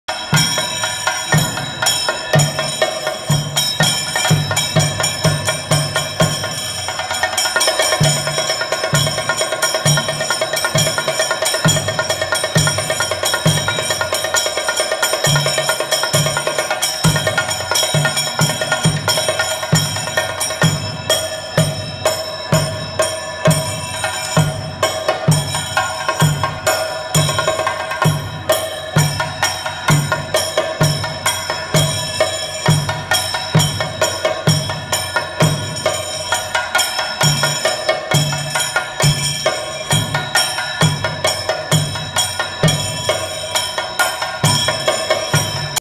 Traditional Kerala welcome at Marriott Whitefield
On the occasion of Onam, Marriott Whitefield introduced an OnaSadya, the traditional feast.